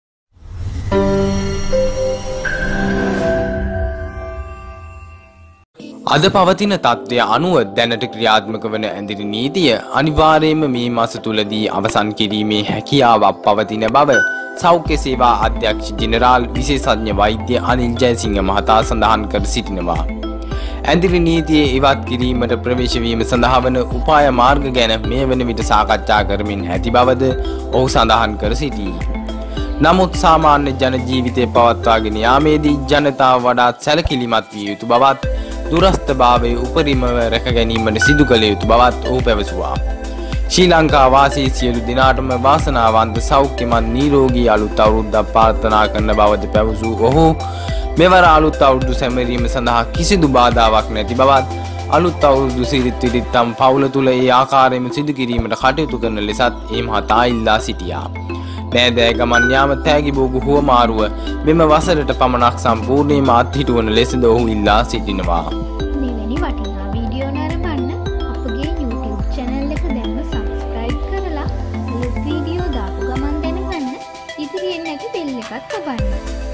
මෙන්න අලුත් අවුරුද්දදේ ඉහලම සෞඛ්‍ය බලධාරියා රටටම දුන්නු සුබ පණිවිඩය
අද පවතින තත්ත්වය අනුව දැනට ක්‍රියාත්මක වන ඇඳිරි නීතිය අනිවාර්යෙන්ම මේ මාසය තුළ අවසන් කිරීමේ හැකියාවක් පවතින බව සෞඛ්‍ය සේවා අධ්‍යක්ෂ ජනරාල් විශේෂඥ වෛද්‍ය අනිල් ජාසිංහ මහතා සඳහන් කරයි.